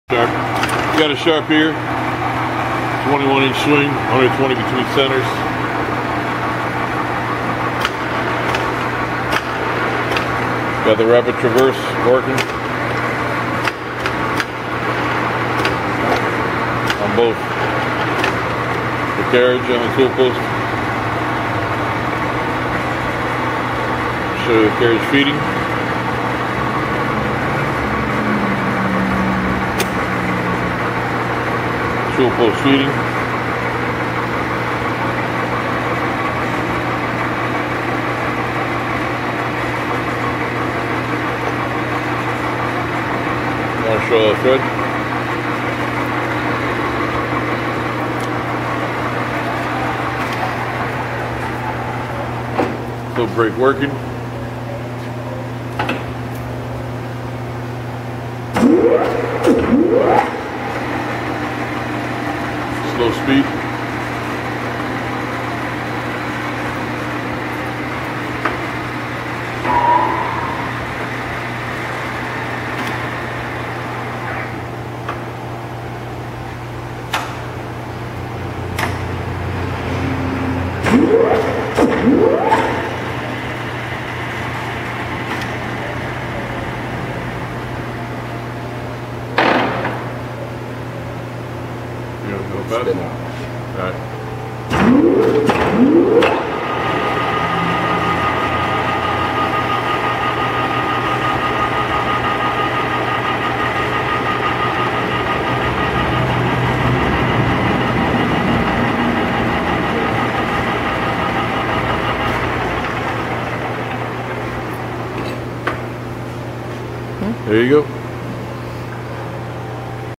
Sharp 21 x 120 Lathe
Testing this lathe for a customer. Showing all is working and in good condiiton. This is a "Sharp" 21 x 120 Lathe

UN TORNO PARALELO UNIVERSAL USADO MARCA “SHARP”
DE CABEZAL ENGRANADO DE CAMBIO RÁPIDO CON ESCOTE
VOLTEO SOBRE LA CAMA  21” / DISTANCIA ENTRE CENTROS 120”